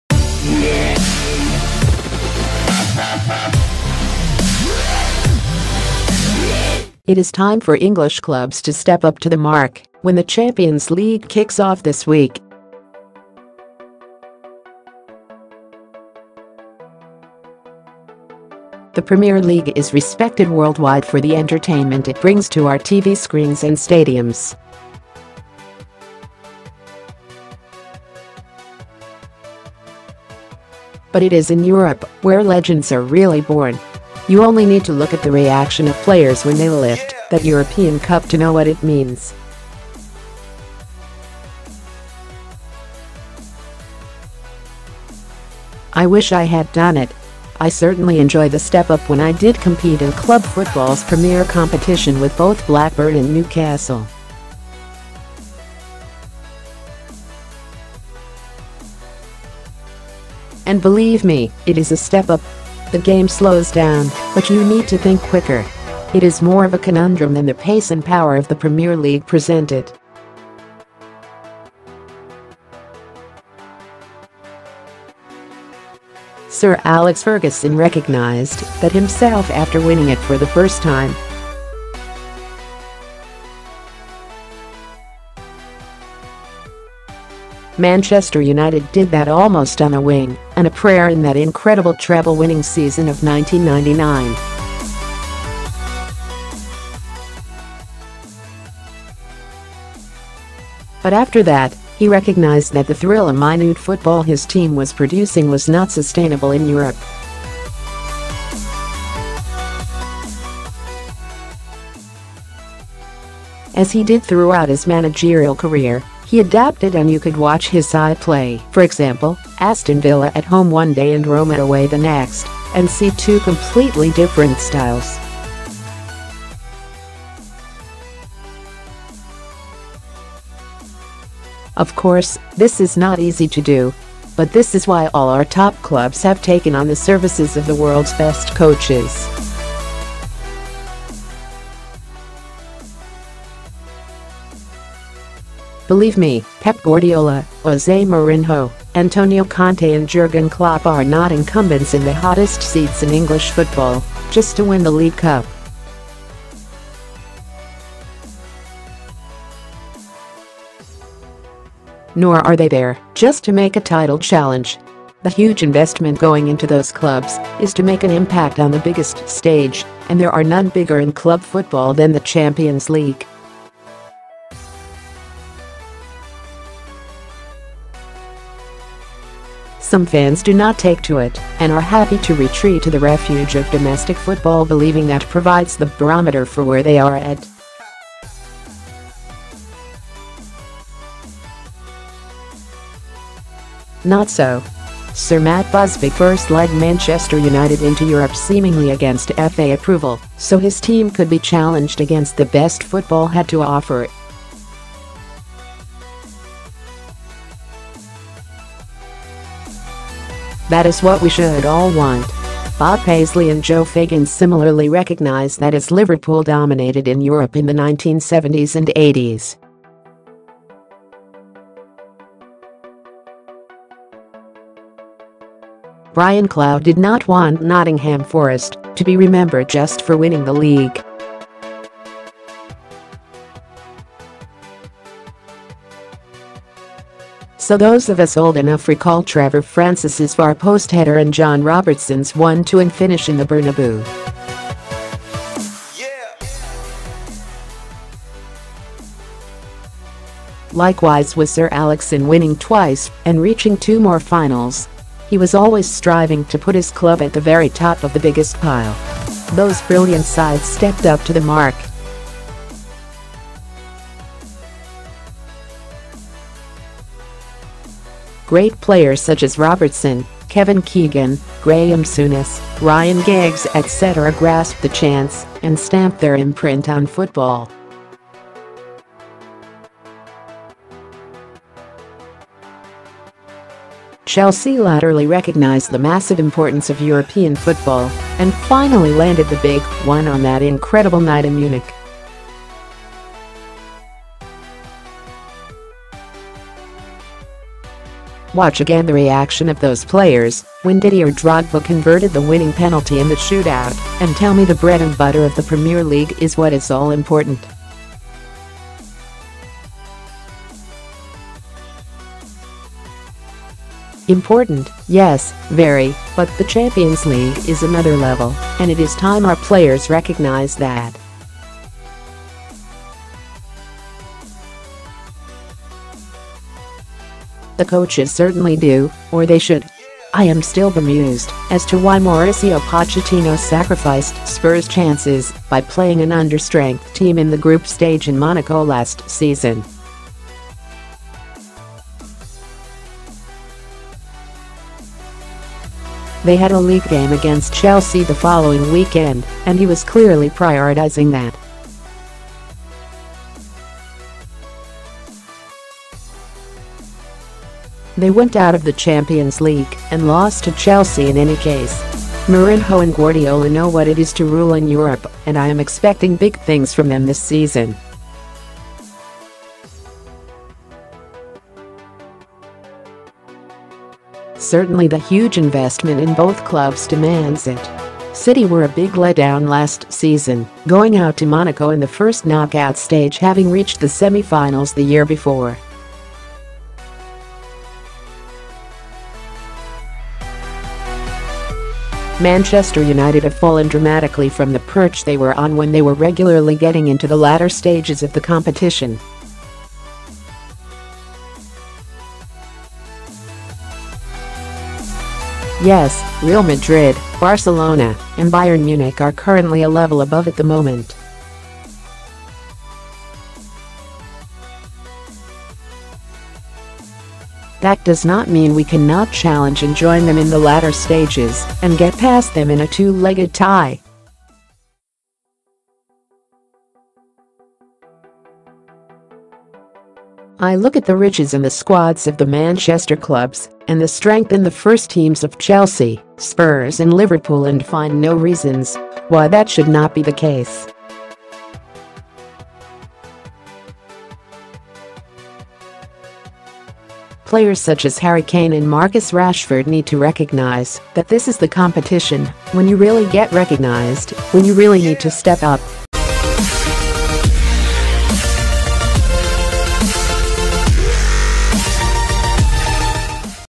Jose Mourinho, Pep Guardiola and Antonio Conte are here to win the Champions League... so it's time
IT is time for English clubs to step up to the mark when the Champions League kicks off this week.

The Premier League is respected worldwide for the entertainment it brings to our TV screens and stadiums.
But it is in Europe where legends are really born.

You only need to look at the reaction of players when they lift that European Cup to know what it means. I wish I had done it.

I certainly enjoyed the step up when I did compete in club football’s premier competition with both Blackburn and Newcastle. And believe me, it is a step up.

The game slows down but you need to think quicker. It is more of a conundrum than the pace and power of the Premier League presented.

Sir Alex Ferguson recognised that himself after winning it for the first time.

Manchester United did that almost on a wing and a prayer in that incredible Treble-winning season of 1999.
But after that, he recognised that the thrill-a-minute football his team was producing was not sustainable in Europe.

As he did throughout his managerial career, he adapted and you could watch his side play, for example, Aston Villa at home one day and Roma away the next and see two completely different styles. Of course, this is not easy to do.

But this is why all our top clubs have taken on the  services of the world’s best coaches.

Believe me, Pep Guardiola, Jose Mourinho, Antonio Conte and Jurgen Klopp are not incumbents in the hottest seats in English football just  to win the League Cup.

Nor are they there just to make a title challenge.
The huge investment going into those clubs is to make an impact on the biggest stage and there are none bigger  in club football than the Champions League.

Some fans do not take to it and are happy to retreat to the refuge  of domestic football believing that provides the barometer for where they are at. Not so.

Sir Matt Busby first led Manchester United into Europe seemingly against FA approval so his team could be challenged against the best football had to offer. That is what we should all want.

Bob Paisley and Joe Fagan similarly recognised that as Liverpool dominated in Europe in the 1970s and 80s.

Brian Clough did not want Nottingham Forest to be remembered just for winning the league. So those of us old enough  recall Trevor Francis’ far-post header and John Robertson’s one-two and finish in the Bernabeu.
Likewise with Sir Alex in winning twice and reaching two more finals. He was always striving to put his club at the very top of the biggest pile. Those brilliant sides stepped up to the mark.

Great players such as Robertson, Kevin Keegan, Graeme Souness, Ryan Giggs etc grasped the chance and stamped their imprint on football.

Chelsea latterly recognised the massive importance of European football and finally landed the big one on that incredible night in Munich.

Watch again the reaction of those players when Didier Drogba converted the winning penalty in the shootout and tell me the bread and butter o